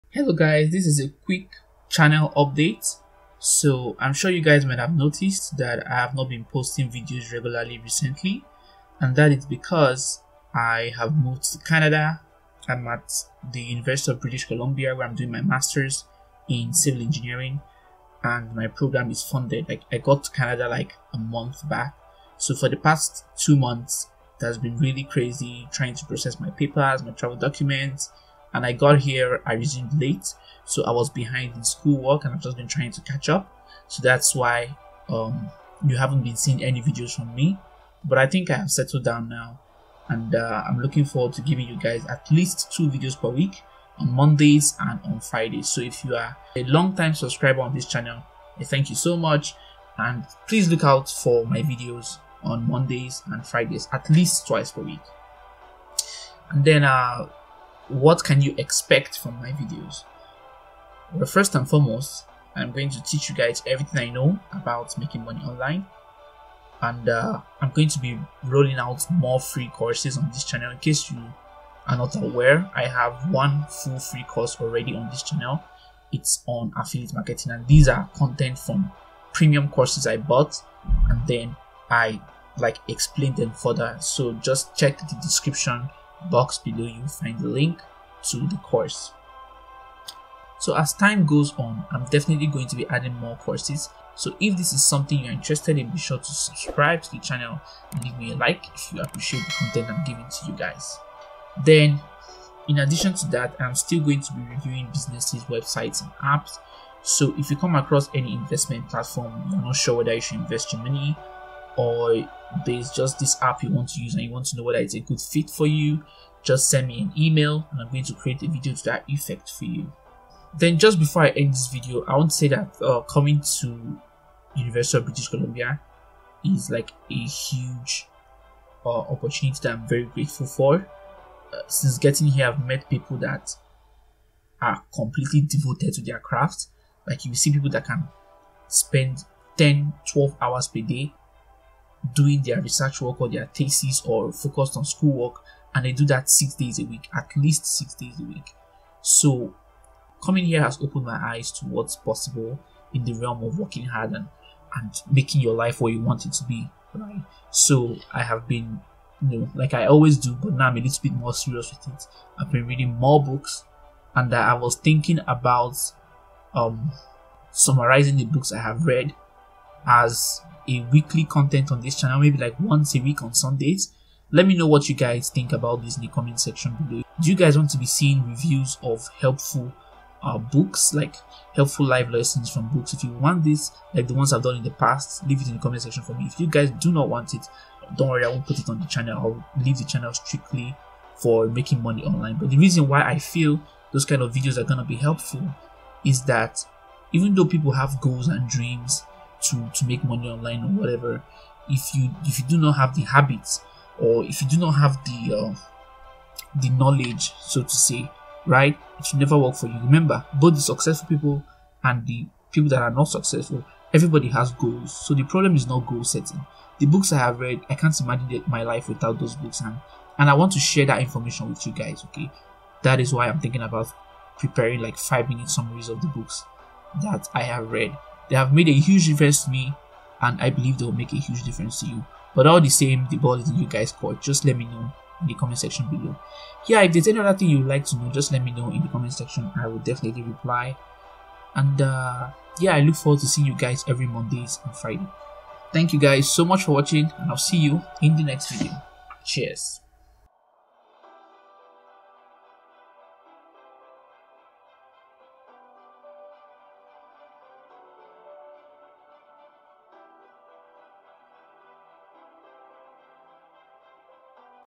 Channel Update!
Hey, I just got to Canada on a full-time program. that is why you haven't been seeing my videos, but I am back now, partially, lol!

Primarily, this channel is all about making money online, but I am thinking about adding reviews of helpful books I have read. Let me know if this is something you guys want to see.

Learn to Make money online with the best work-from-home jobs.

Marissa Romero, Andrei Jikh, Investor, TheProdigy, Daniel's Hustle